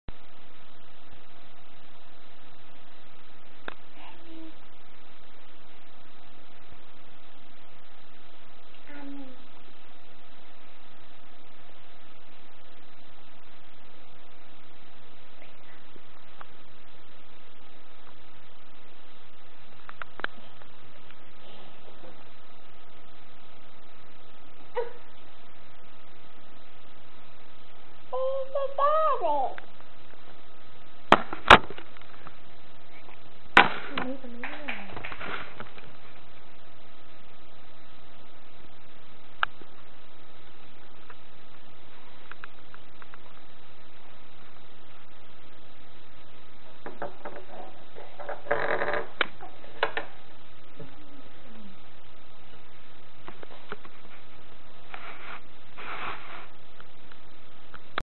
Emily falling alseep at the table
This was taken about 10 minutes ago, no stopping her when she is tired!!